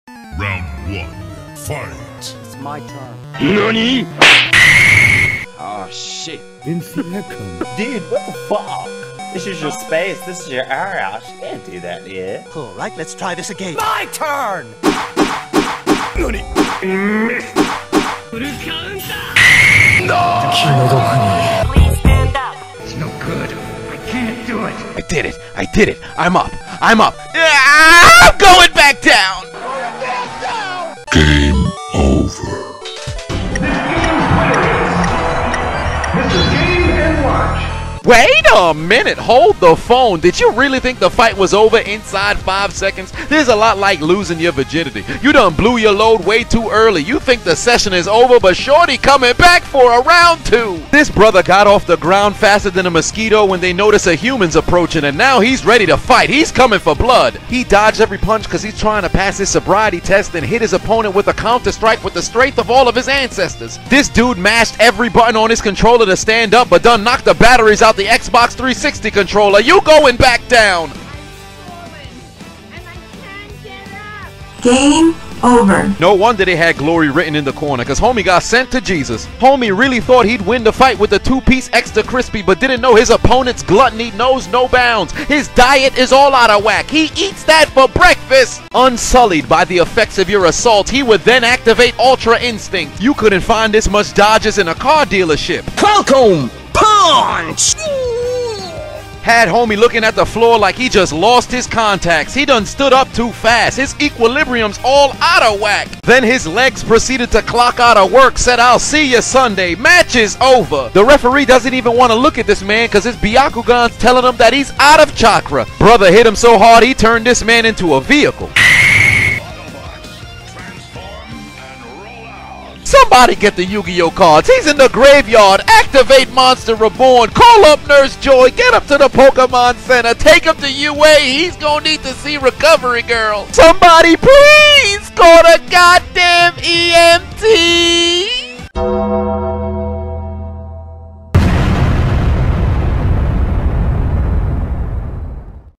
Boxer Gets Knocked Down Then Activates Ultra Instinct & Uses Full Counter | Chiseled Adonis
BIG NEWS! I'm now on Cameo!

Your names will be included in the end of the videos!!